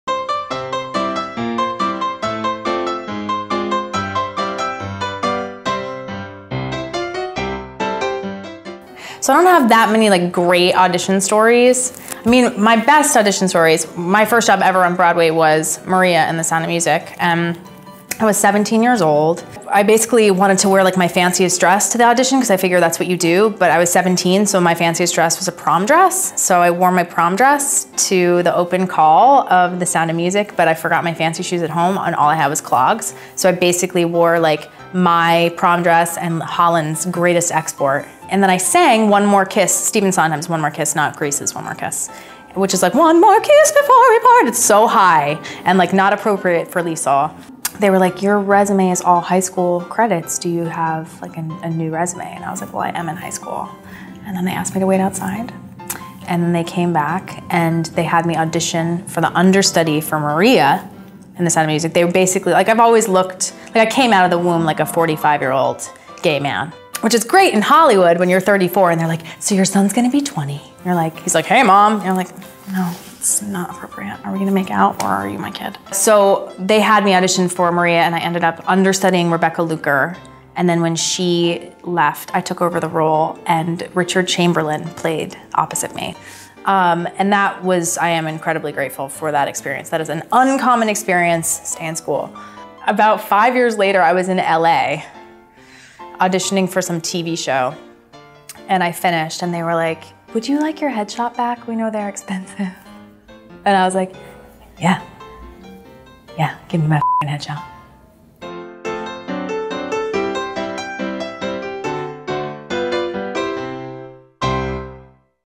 I Hope I Get It: Stories From the Audition Room With Laura Benanti
Long before the The Sound of Music Live! was a gleam in Carrie Underwood's eye, Benanti was 17 and auditioning for The Sound of Music on Broadway.

In TheaterMania's new video series, "I Hope I Get It: Stories From the Audition Room," your favorite Broadway performers divulge horrifying audition secrets and share their hard-earned insights.

At just 17 years old and wearing her high-school-prom best, Tony winner Laura Benanti was auditioning for (and bagging) her first Broadway role: Maria in The Sound of Music. In this video, Benanti remembers that experience and one slightly-less-glamorous audition.